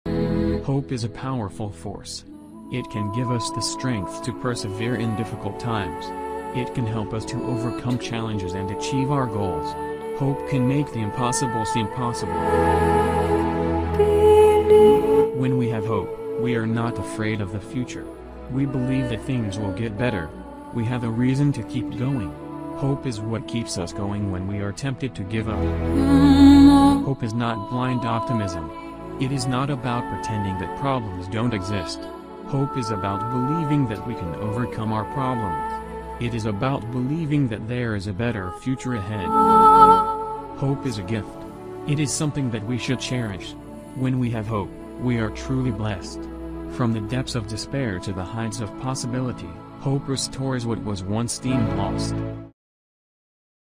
Hope is a powerful force
Hope is a powerful force. It can give us the strength to persevere in difficult times. It can help us to overcome challenges and achieve our goals. Hope can make the impossible seem possible.

When we have hope, we are not afraid of the future. We believe that things will get better. We have a reason to keep going. Hope is what keeps us going when we are tempted to give up.

Hope is not blind optimism. It is not about pretending that problems don't exist. Hope is about believing that we can overcome our problems. It is about believing that there is a better future ahead.

Hope is a gift. It is something that we should cherish. When we have hope, we are truly blessed. From the depths of despair to the heights of possibility, hope restores what was once deemed lost.